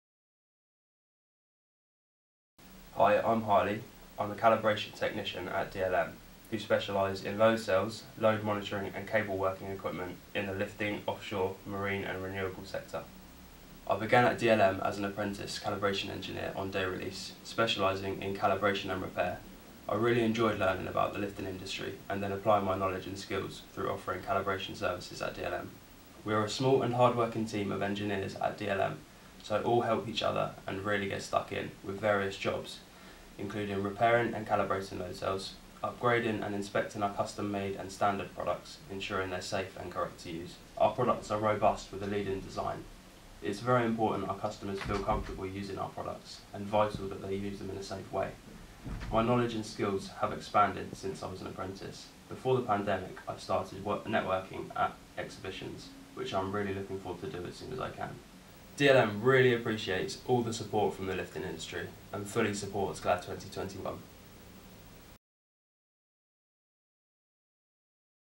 DLM Supports GLAD 2021
Harley our Calibration Technician gives us a short introduction into how he started at DLM, and what he thinks about the Lifting industry on Global Lifting Awareness Day 2021.